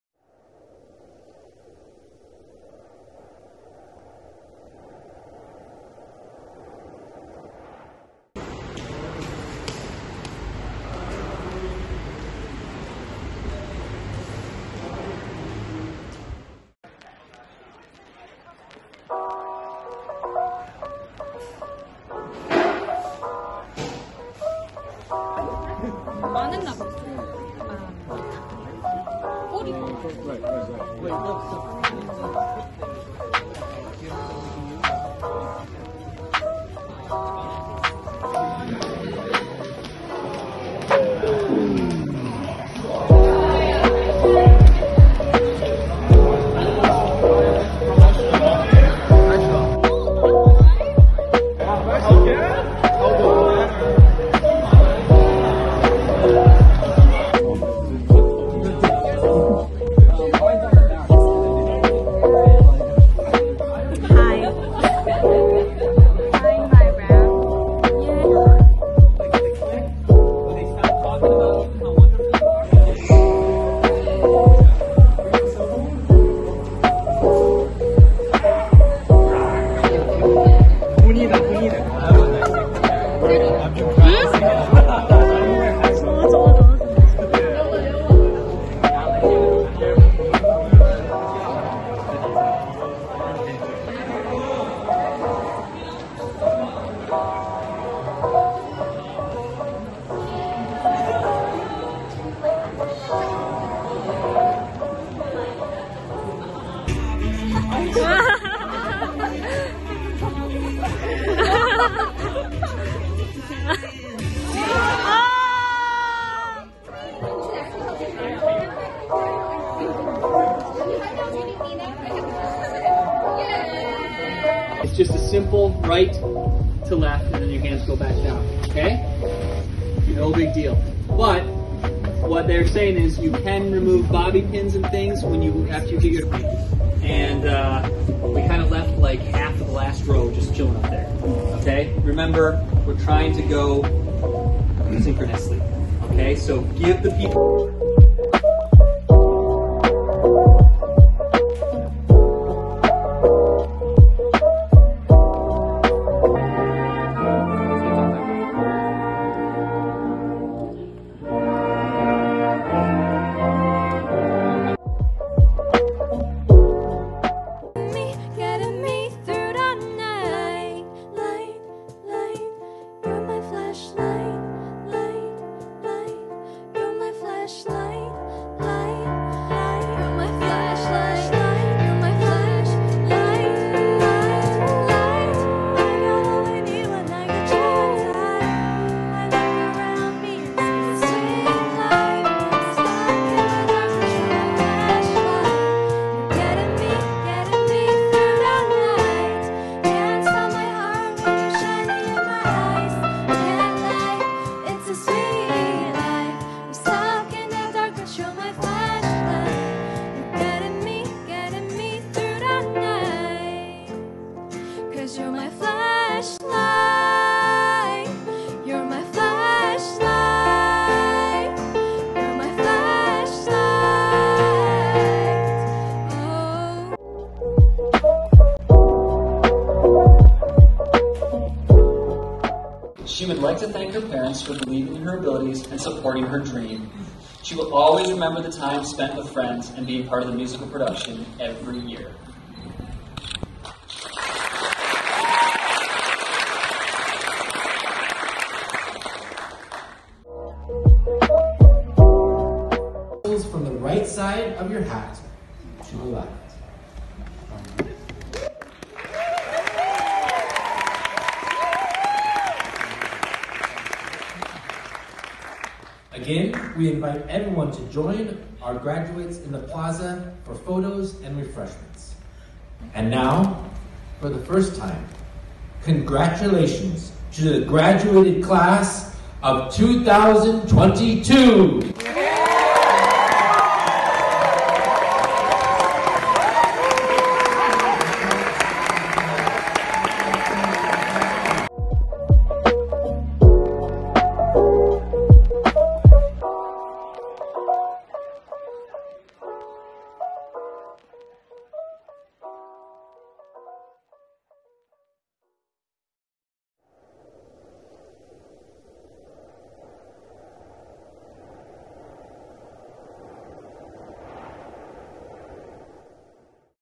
A Playlist To Remember | High School Graduation | Life Vlog
Hi everyone! It's Rosey here ;) If the video quality looks a bit weird... that's because I filmed it with my phone (￣▽￣)
*A Playlist To Remember is the theme of our graduation
Special thanks to Lucy J, Anika K